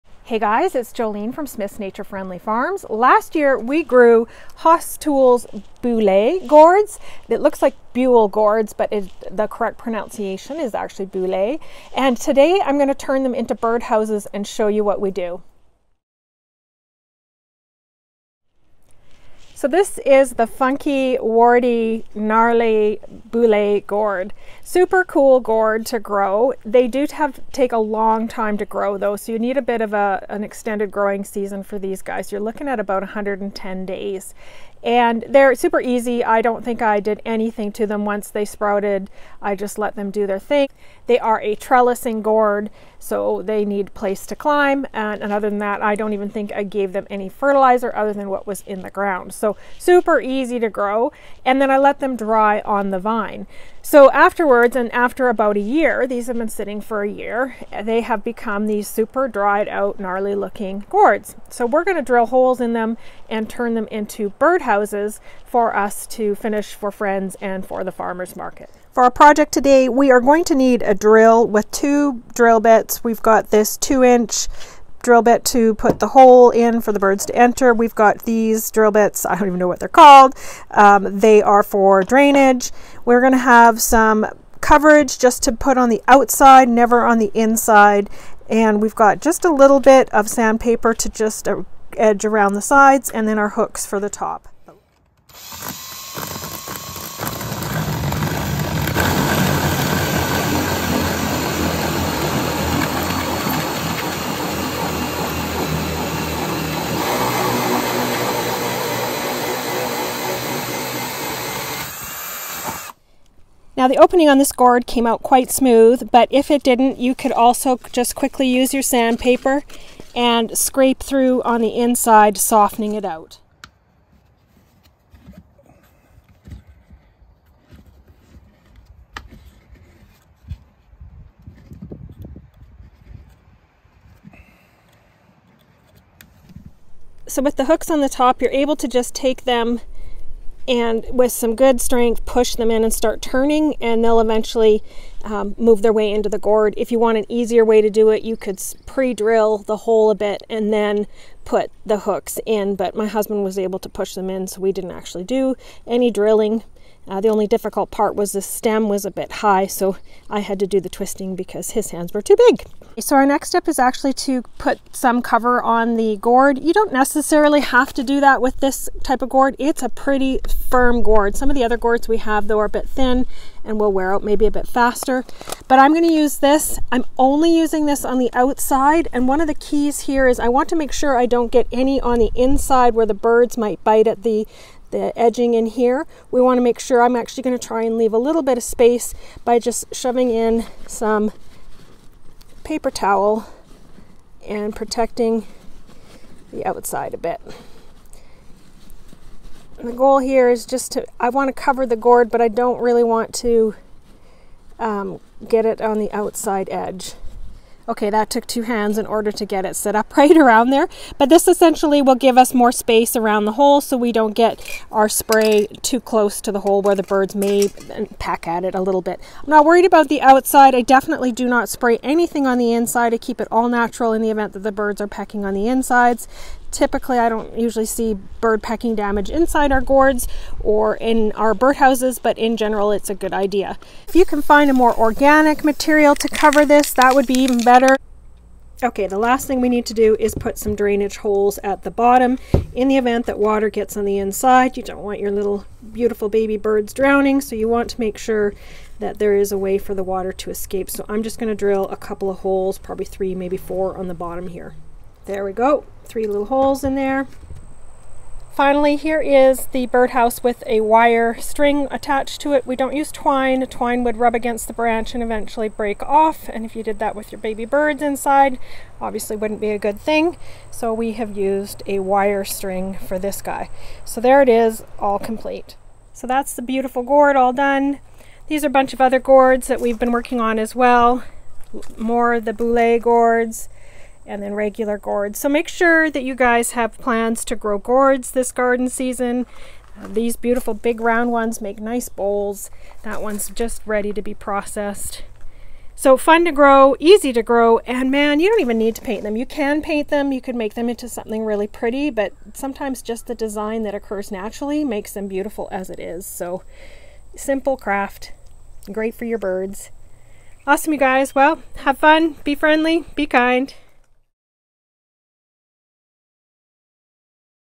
Grow and make the BEST bird house.
We have turned our gourd into a gnarly bird house!

We appreciate you joining us. Be friendly. Be kind.